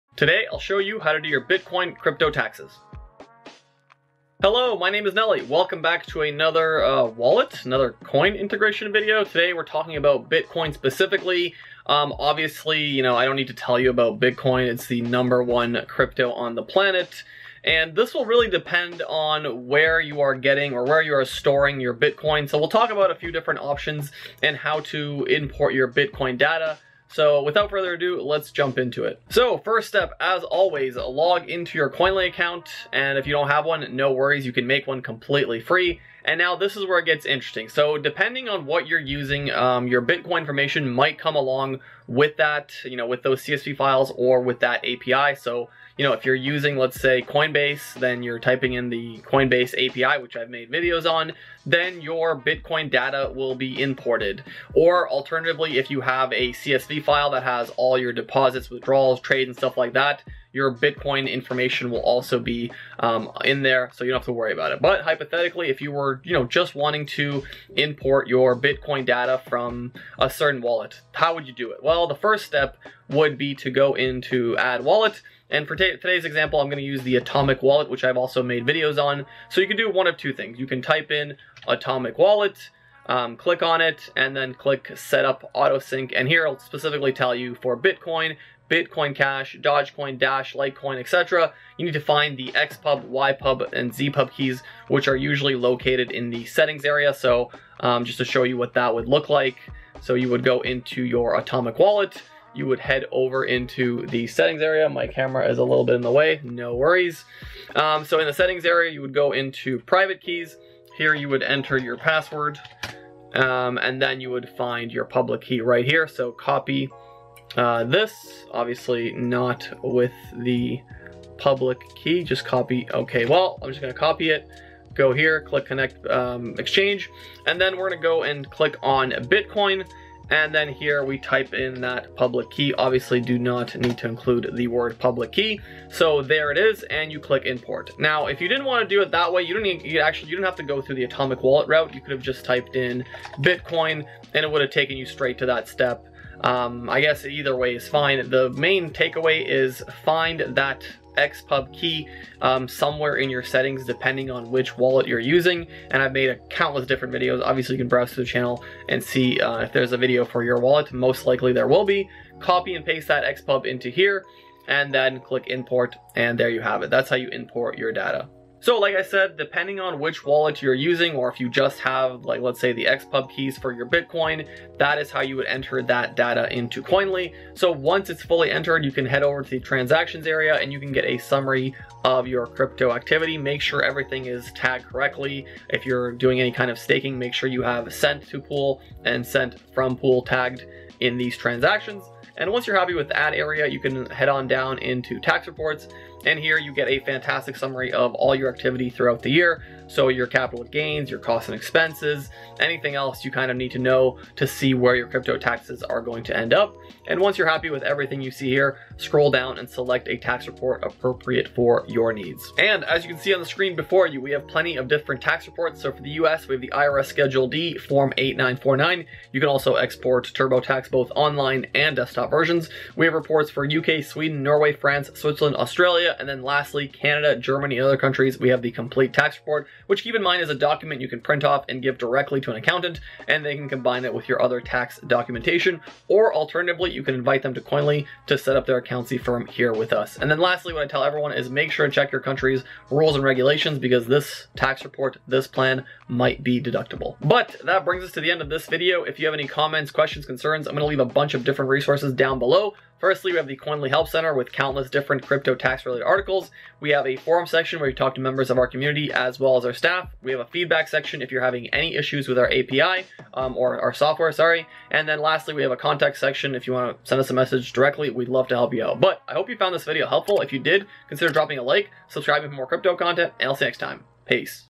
How To Do Your Bitcoin Crypto Tax FAST With Koinly - 2022
Trading with Bitcoin  ? Not sure how to do your crypto tax? Worried about the IRS or your country’s tax authority? Relax, we've got you! Watch and learn with Koinly.

Using Bitcoin and wondering how to do your Bitcoin crypto taxes before the IRS, HMRC, CRA, or the ATO tax deadline? It’s really easy! Today I’ll show you have to quickly import your Bitcoin transactions into Koinly -  by importing different CSV files into Koinly. Koinly then calculates your cryptocurrency taxes and generates a fully compliant tax report that you file with yourself, or send to your crypto tax accountant. Let’s get your crypto tax sorted!

[00:00] Introduction
[00:50] Integration
[4:00] How To Download Tax Reports

Want more info? Check out our step-by-step guide on how to get your Bitcoin crypto tax done with Koinly